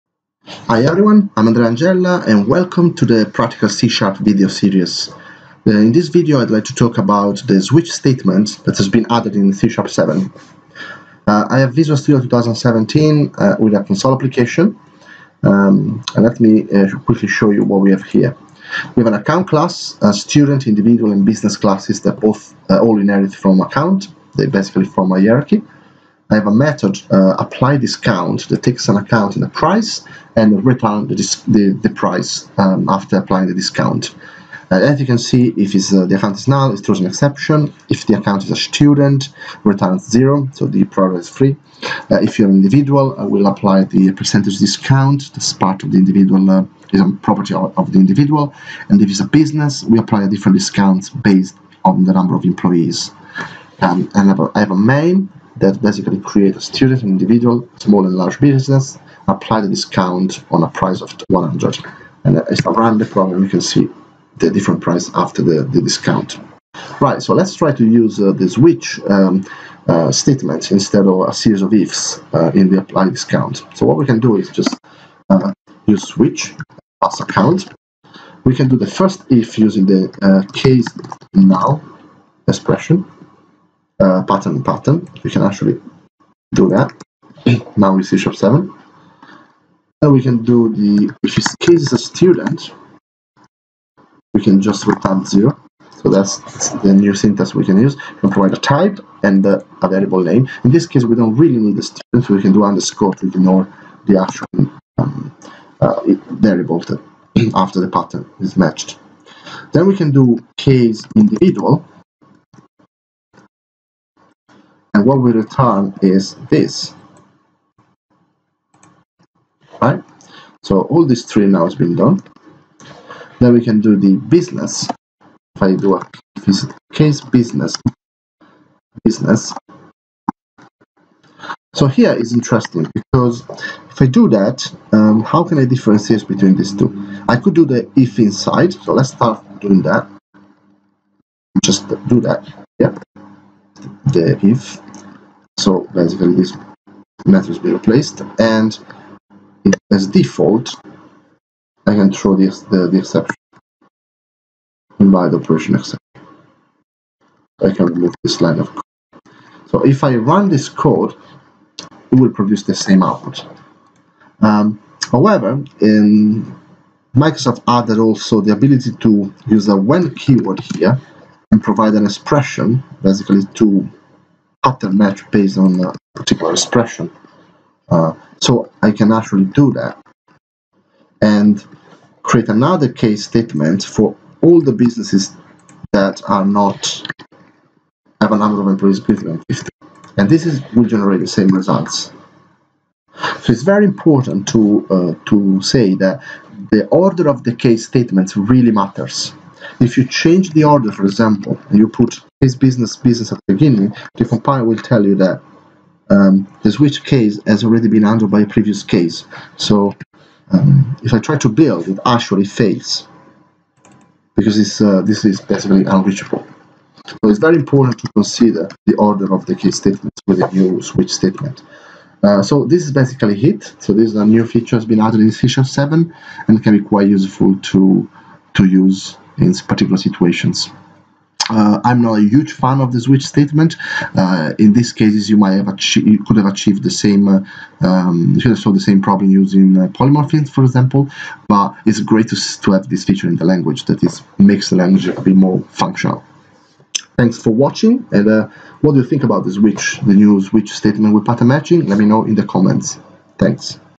Practical C# - Switch Statement with Pattern Matching in C# 7
Learn how to refactor a series of ifs using the improved switch statement with pattern matching in C# 7.

Enjoy the video and leave your questions and comments below.